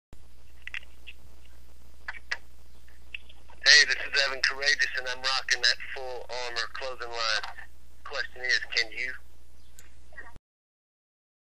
Full Armor Clothing commercial with Evan Karagias
Former WCW star Evan Karagias sporting Full Armor Clothing. check out